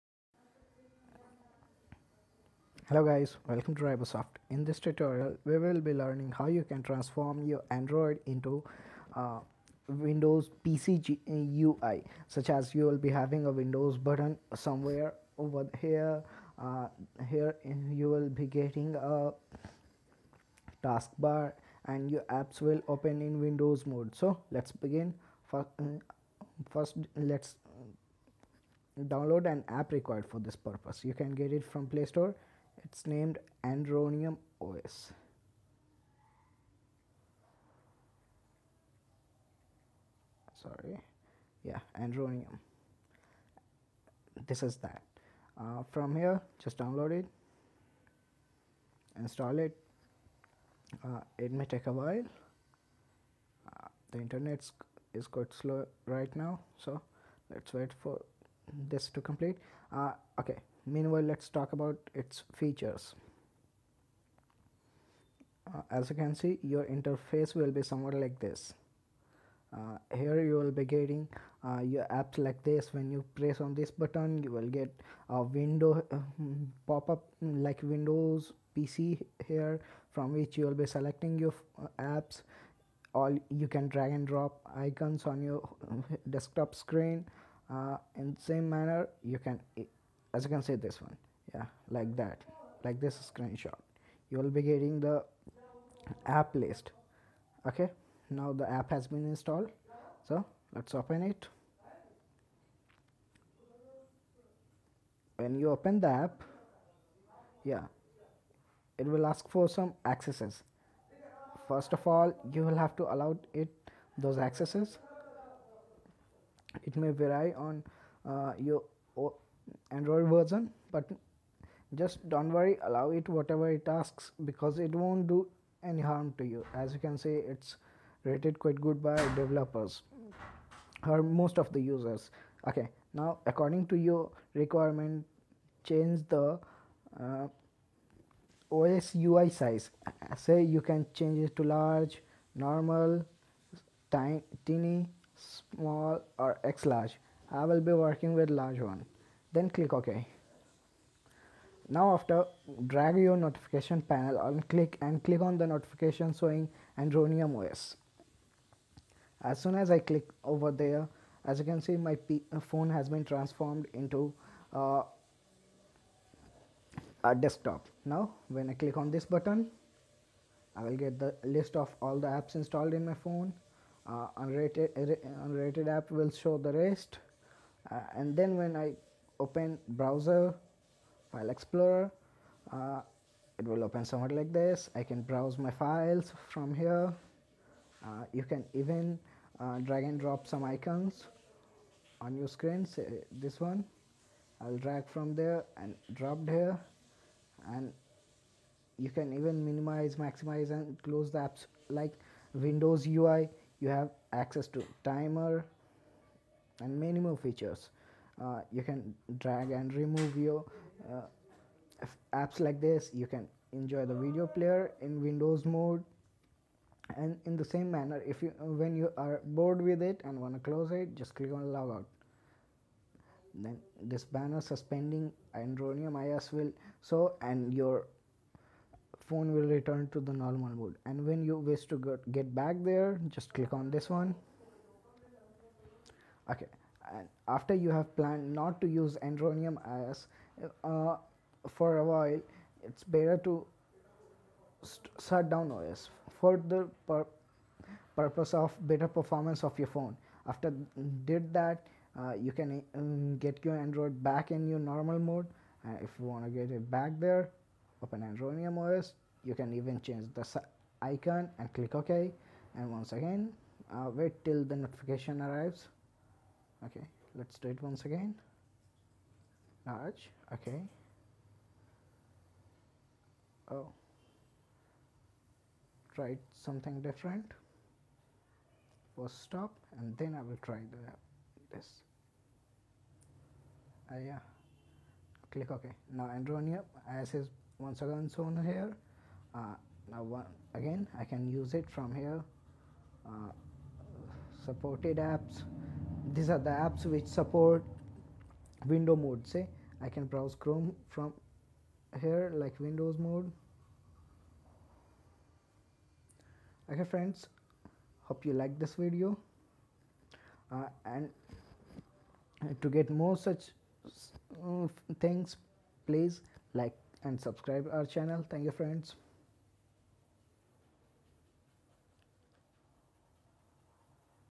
How to Convert Android In Windows PC
It's a tutorial about how to Convert Android In Windows PC It's a cool method to transform your Android phone into a PC ie you can use app Andronium OS to try this cool thing.